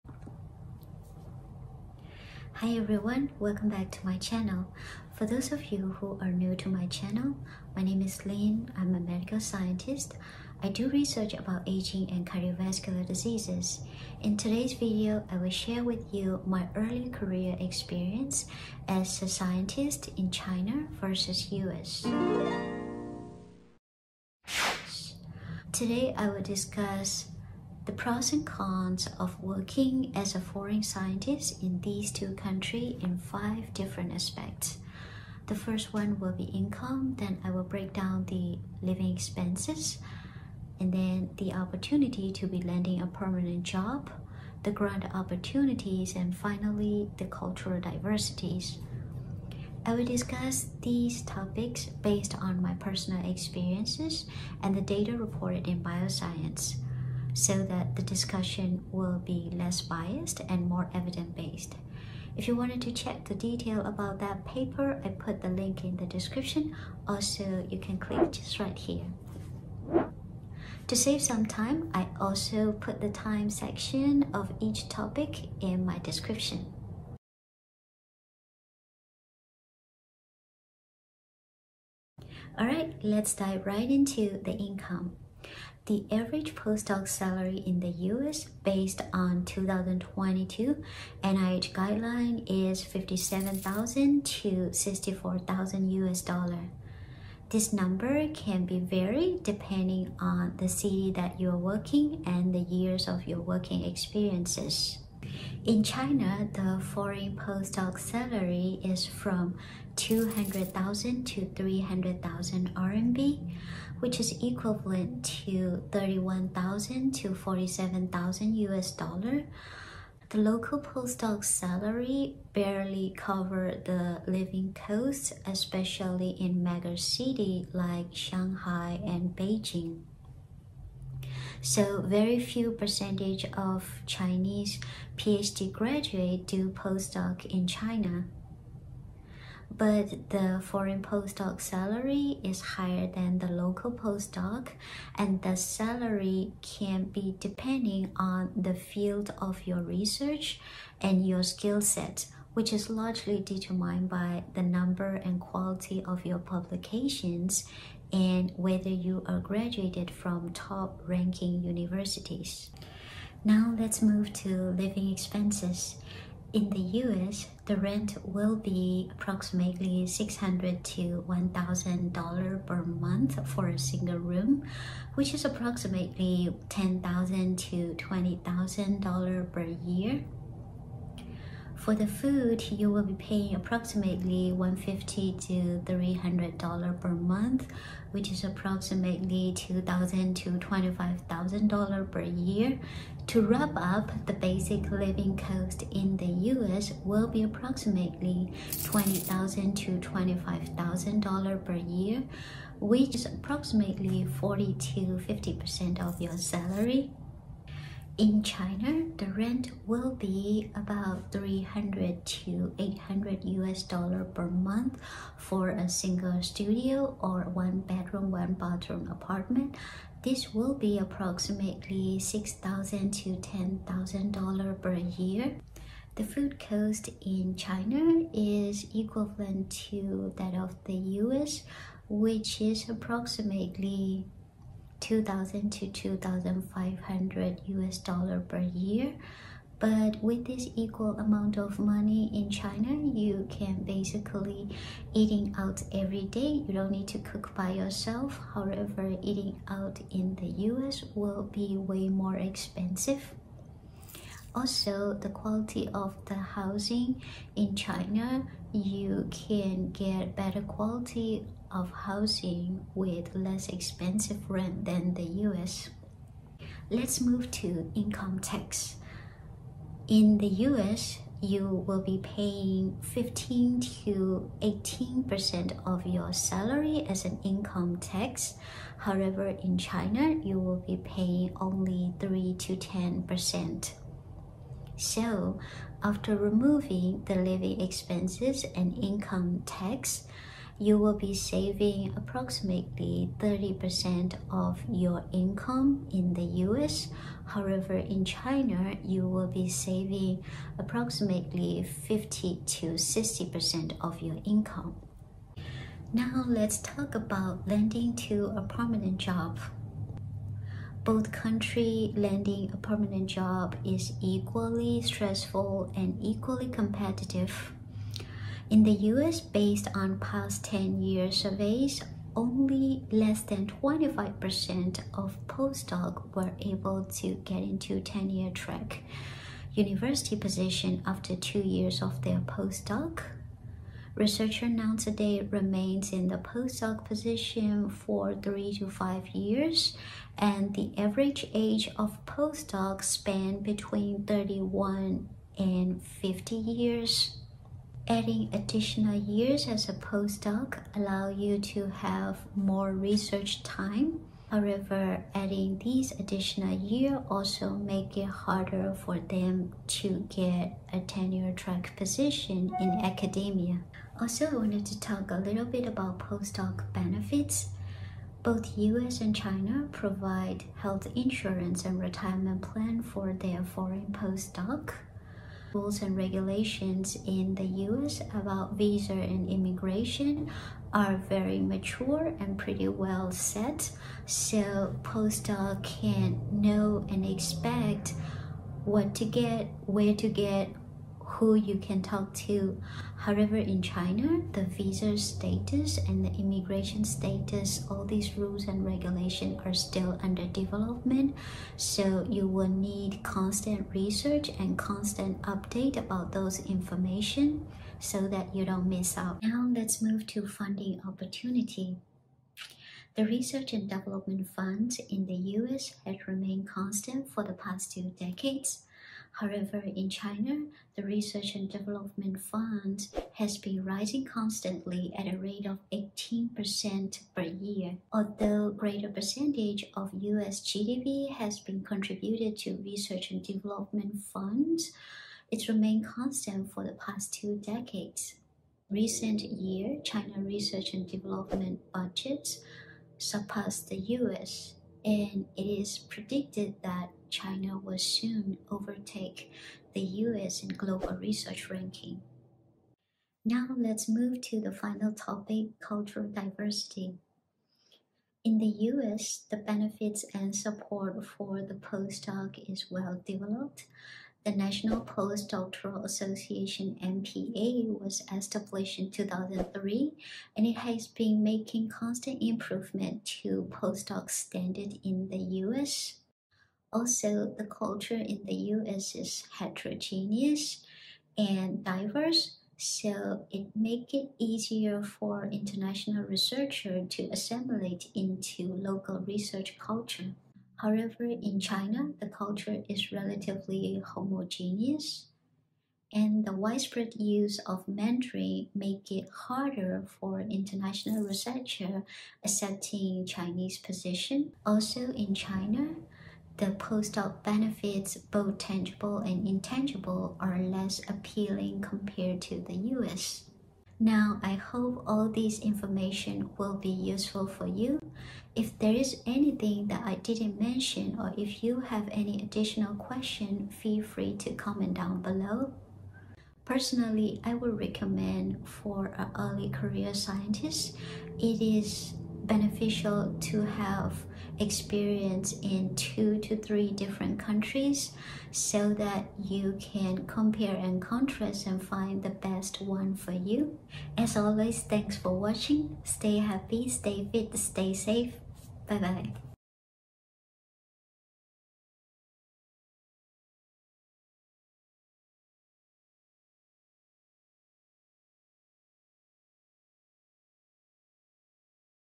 Working as a Foreign Medical Scientist in China vs USA | Early Career Scientist | Pros and Cons
Hi everyone, in this video, I will share with you my experiences on working as an early career scientist in China vs USA along with data reported in Bioscience. I will make sure to get back to you. Peace!

00:00 Intro
01:24 Income
02:45 Living expenses
05:07 Lending to a permanent job
06:05 Postdoc benefits
06:49 Funding opportunities
07:35 Cultural diversity

Links:
Postdocs in Science: A Comparison between China and the United States